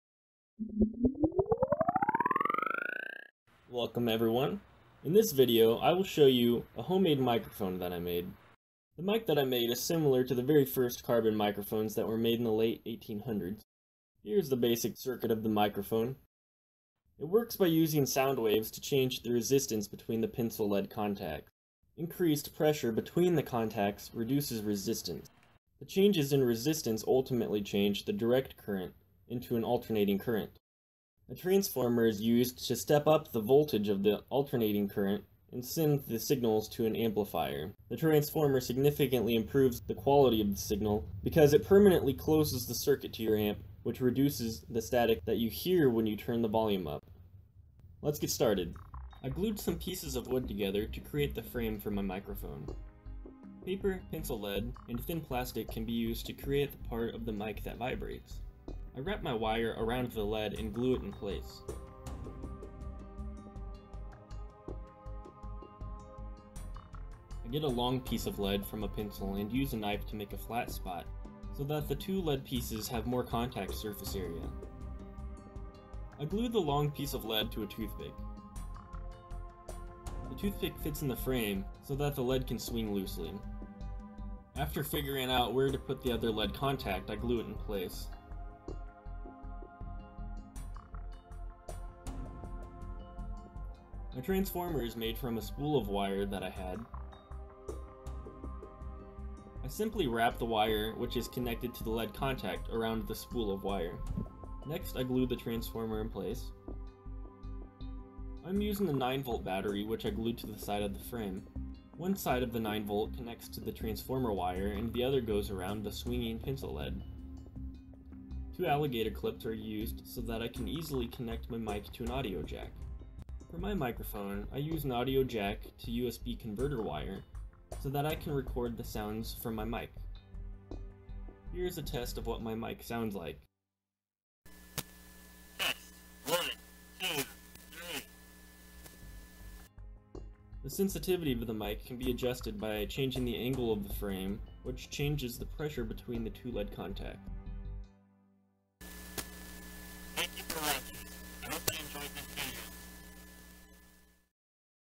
How to make a Microphone
This video explains how to make a carbon microphone using household items including:
Pencil Lead
Battery
Wood
Wire
Paper
Plastic Lid
Hot Glue

If you liked this video, you can help me make more by donating to my
Bitcoin Address: 1HrTpm9Vh3S8YSAgJ5YKiYD5LKChov1BKZ

Recorded by Sonidor